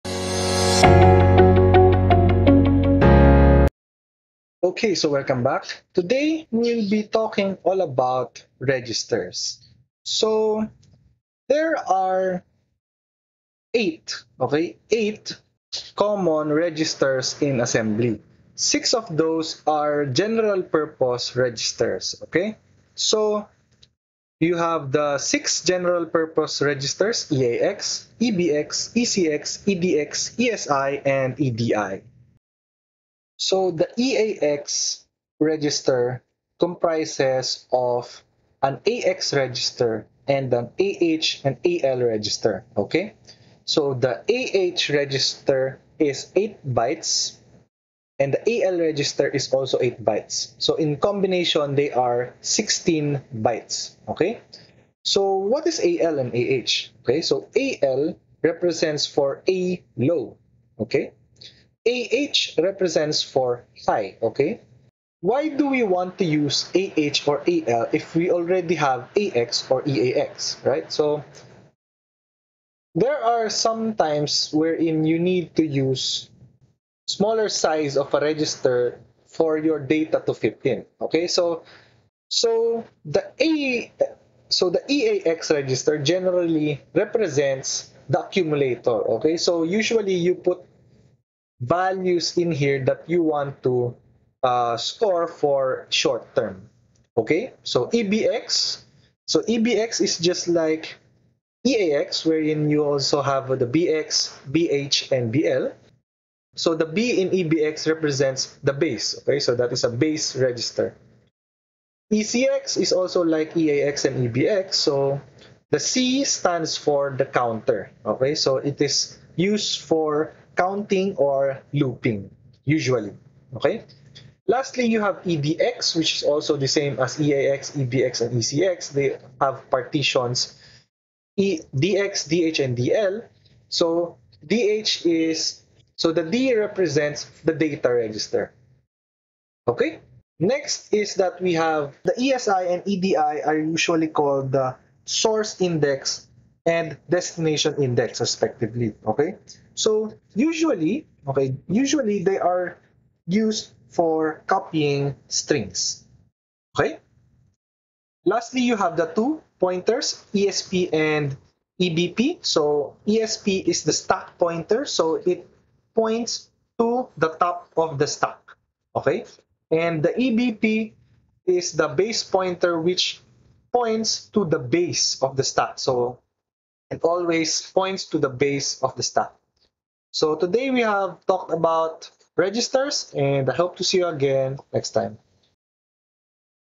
Registers | MASM32 Assembly Programming
In this video, you will be learning about masm32 assembly registers